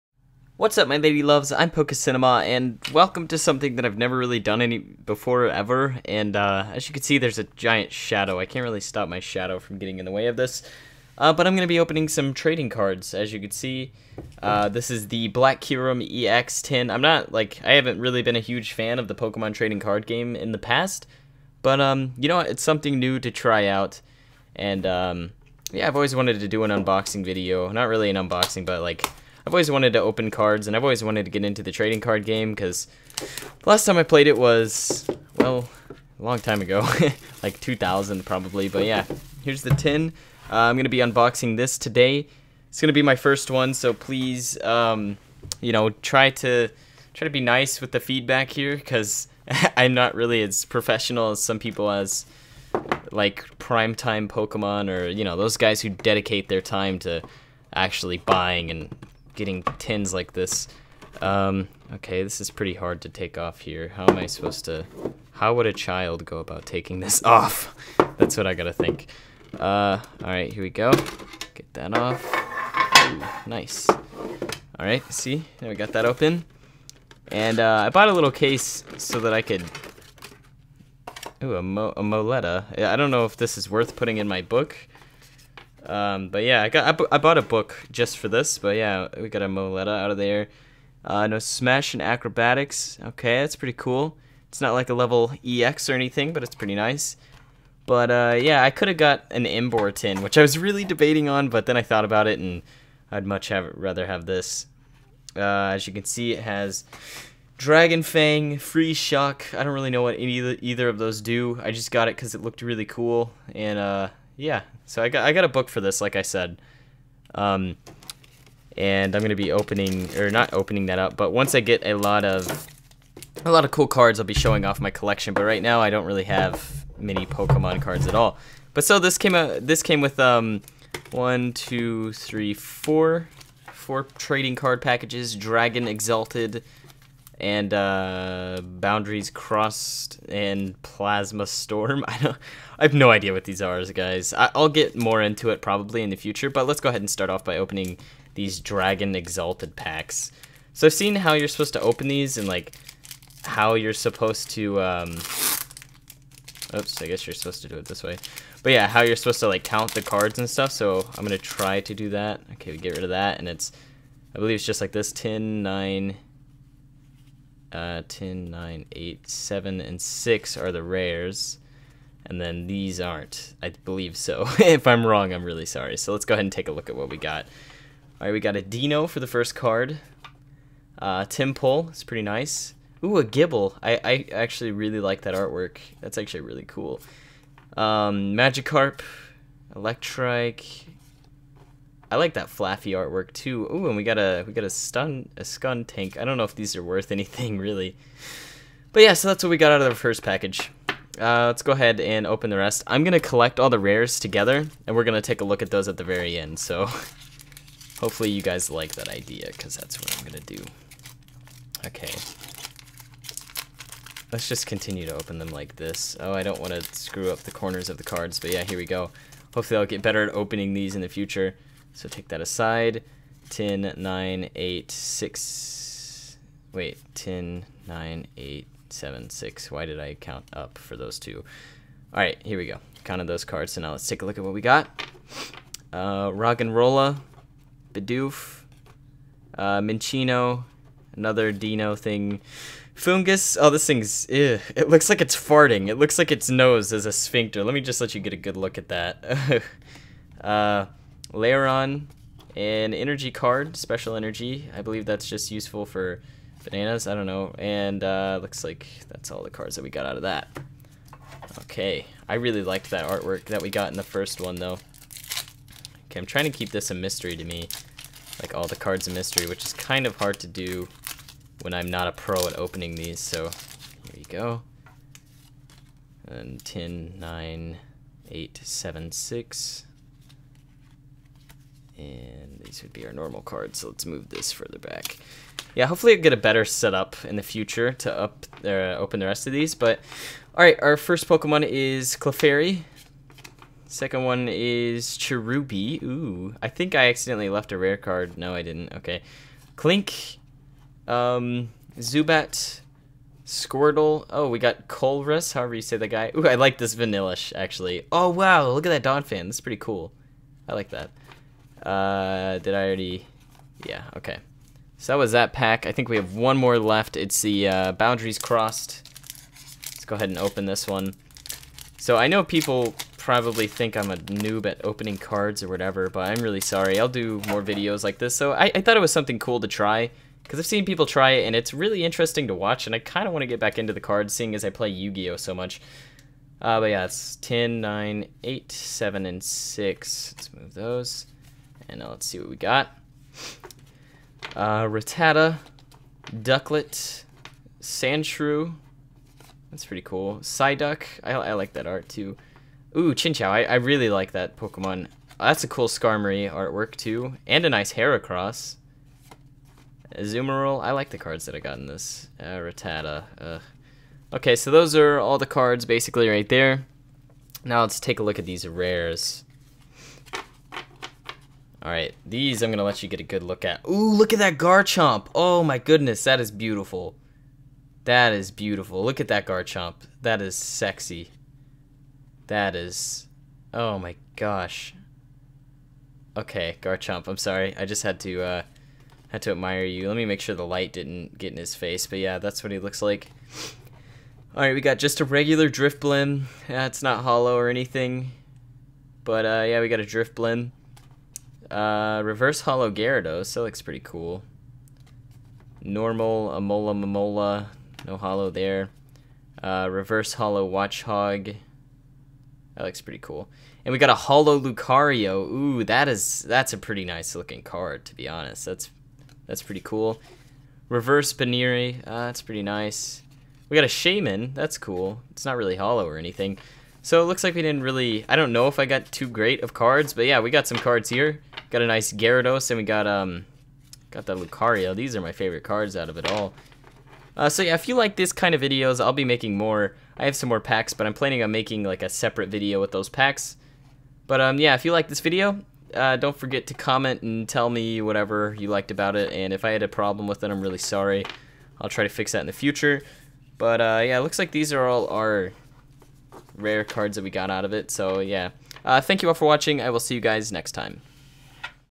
Opening Black Kyurem EX Tin!
Hey guys I just wanted to try something new! If you like these unboxing videos be sure to let me know by leaving a like, and a comment!